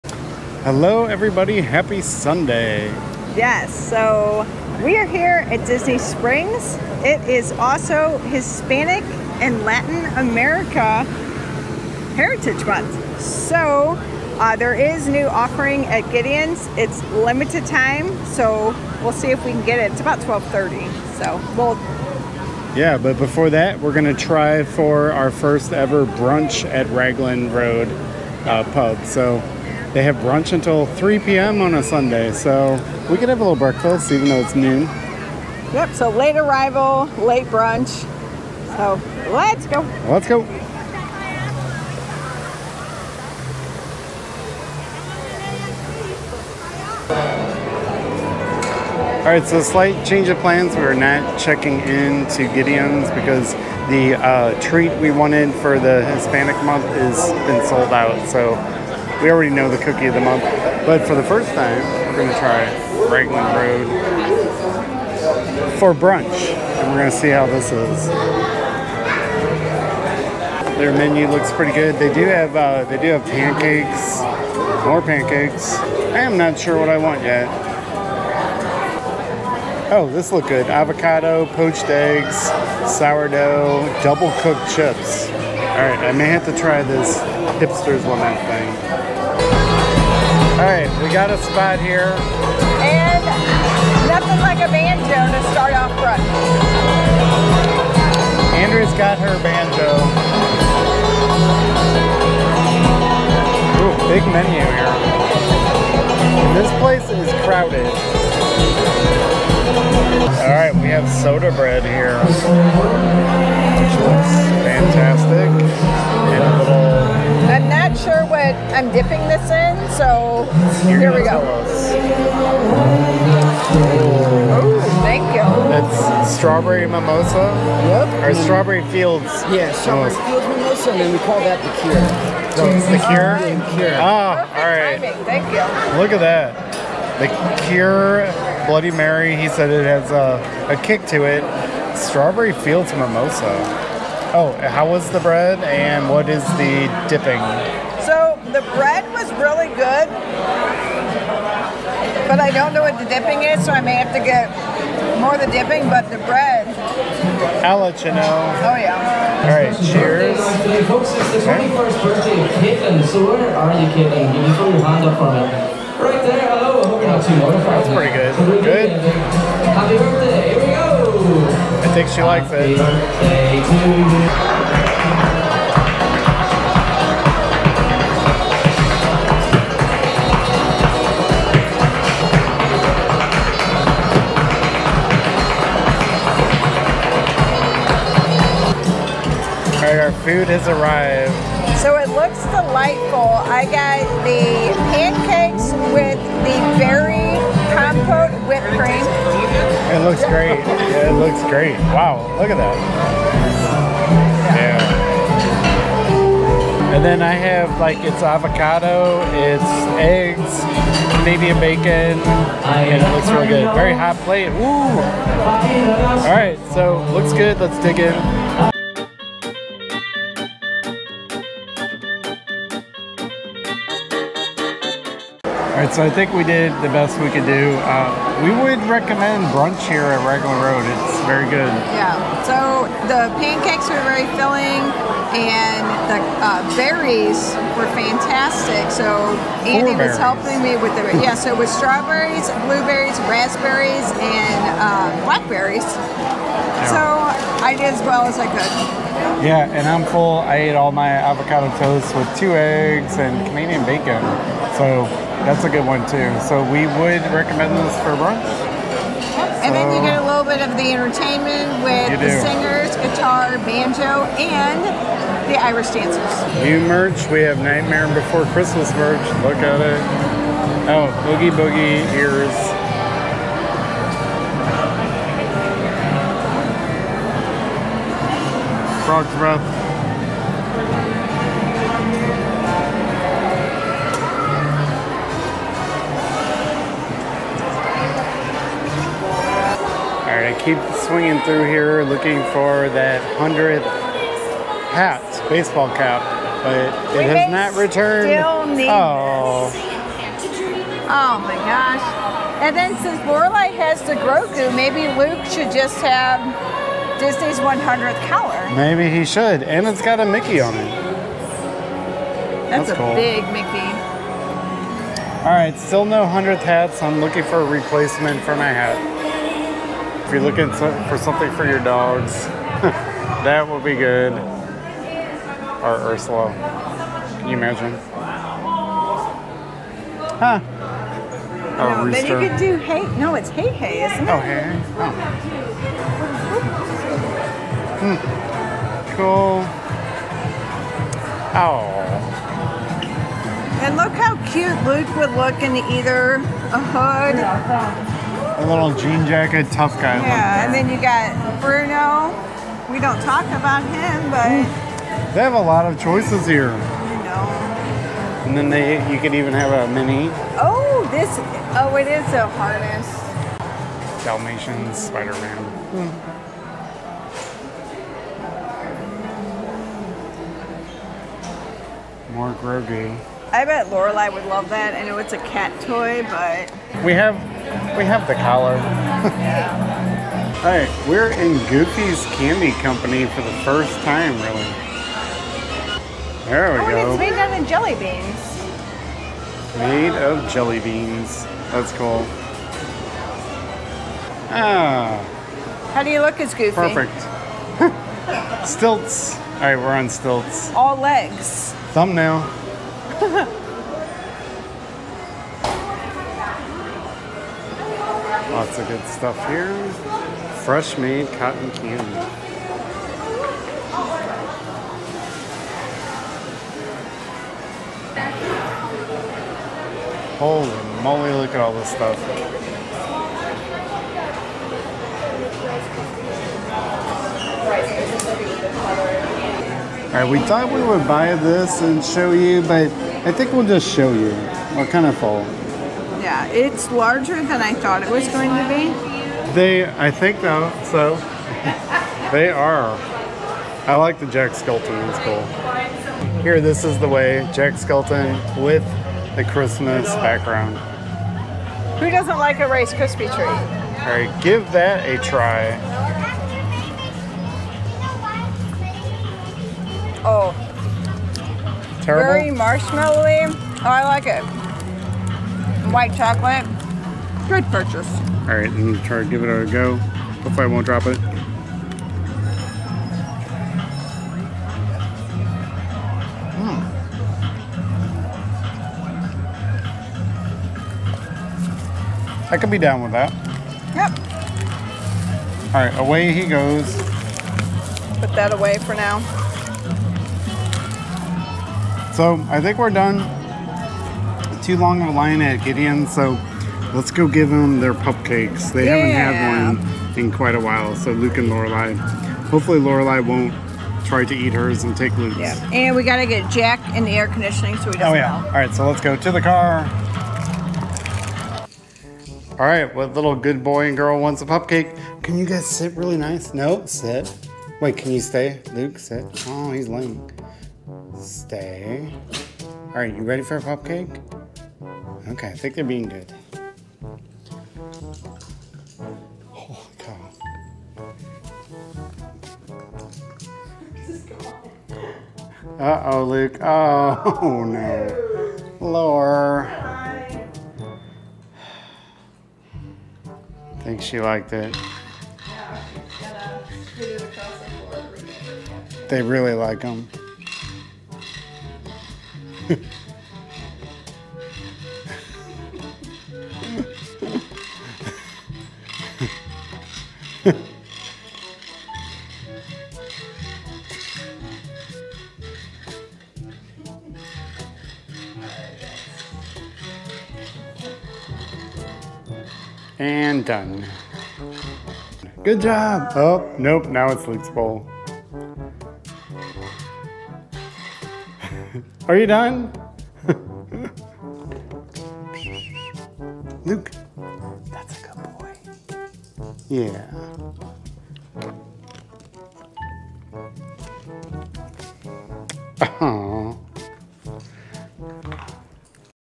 Brunch at Raglan Road Irish Pub in Disney Springs at Walt Disney World | Disney Dining Review
Today we have brunch at Raglan Road Irish Pub and Restaurant in Disney Springs!! This is our first ever brunch at one of the most famous restaurants in Disney Springs - Raglan Road! An authentic Irish Pub with incredible food, live entertainment, and several bars, Ragland Road never disappoints. We were wanting to try a new brunch spot (new to us!) and Ragland Road's brunch did not disappoint! Enjoy!

Want to book a cruise or land adventure? Contact Andrea